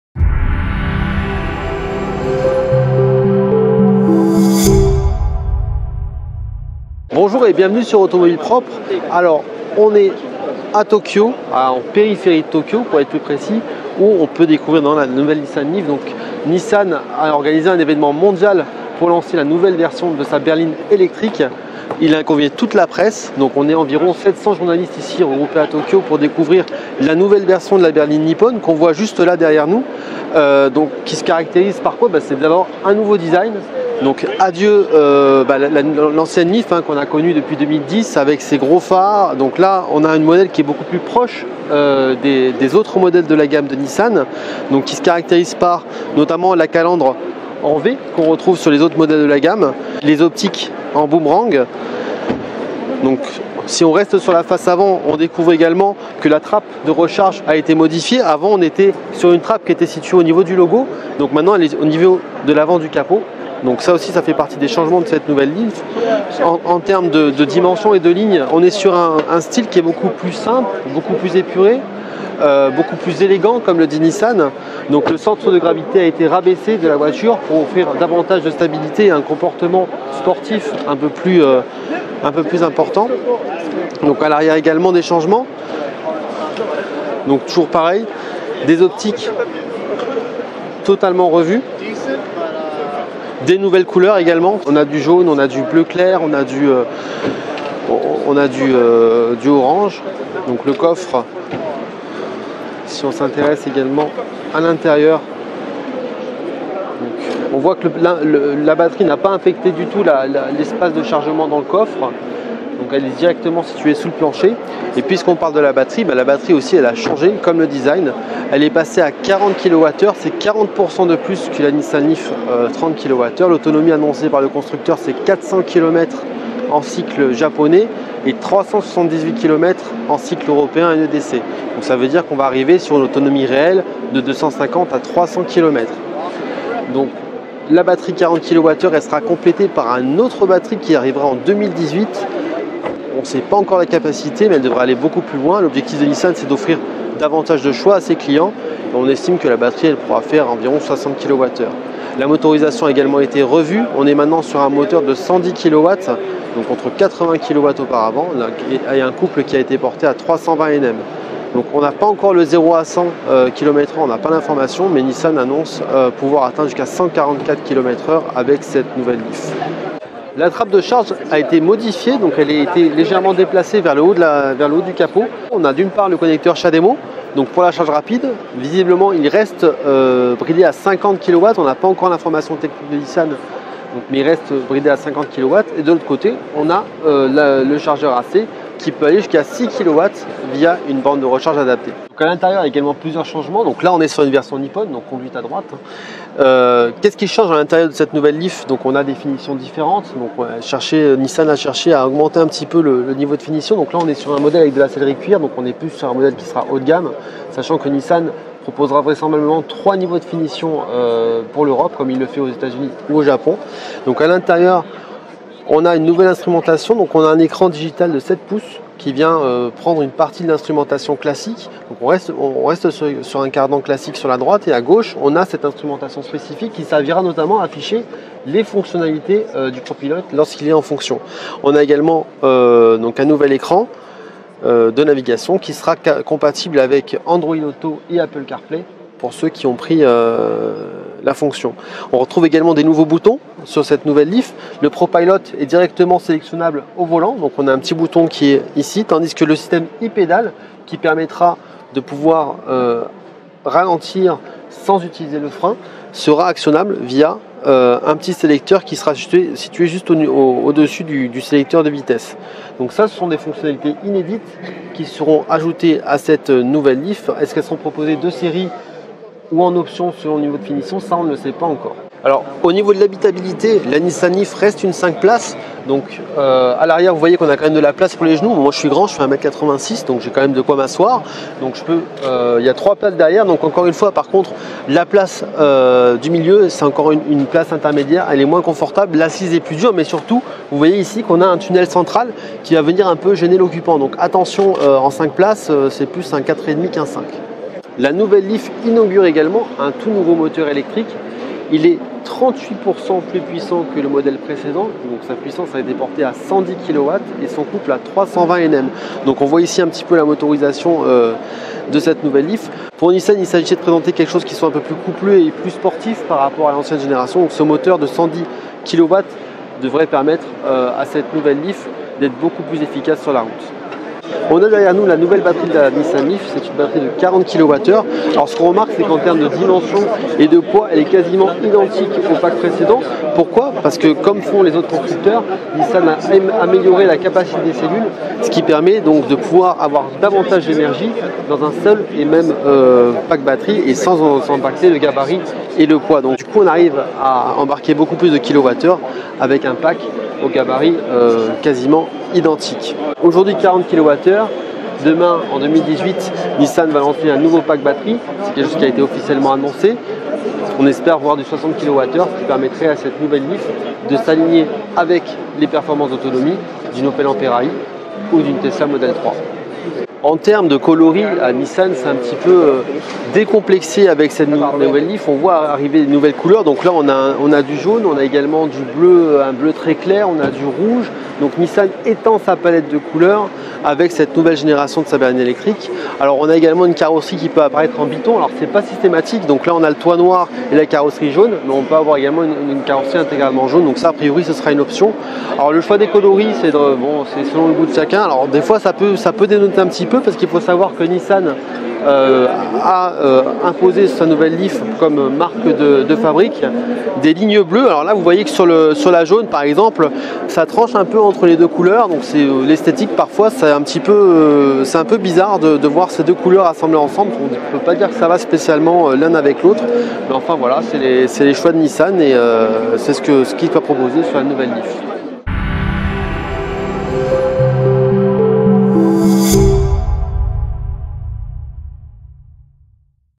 Nissan LEAF 2018 : tout ce que vous devez savoir !
Une présentation complète de la nouvelle Nissan LEAF 2018 filmée à Tokyo.